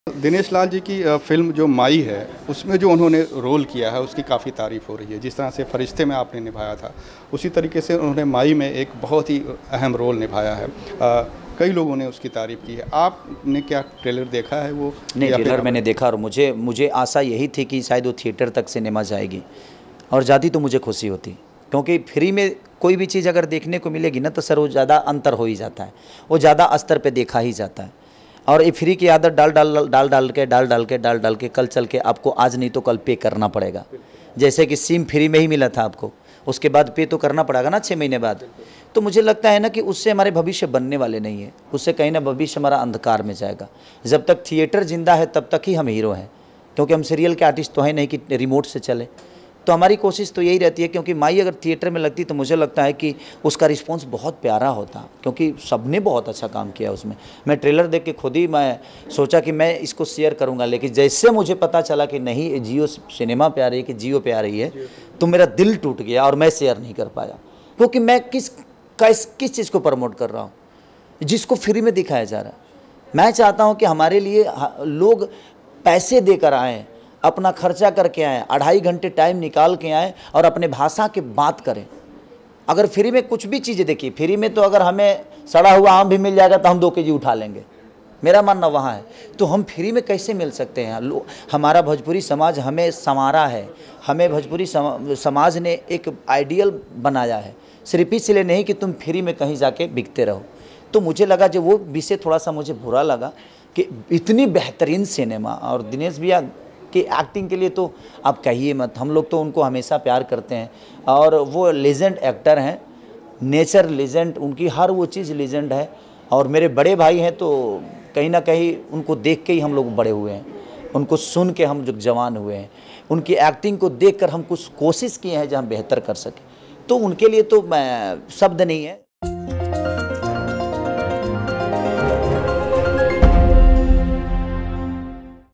Khesari Lal Yadav ने इसलिए नहीं किया Dinesh Lal Nirahua की फिल्म "माई" का प्रमोशन | Lehren Bhojpuri
भोजपुरी सिनेमा के ट्रेंडिंग स्टार खेसारी लाल यादव ने दिनेश लाल यादव निरहुआ की फिल्म माई की जमकर तारीफ की। पर जियो सिनेमा पर रिलीज होने की वजह से उन्होने इस फिल्म को प्रमोट नहीं किया।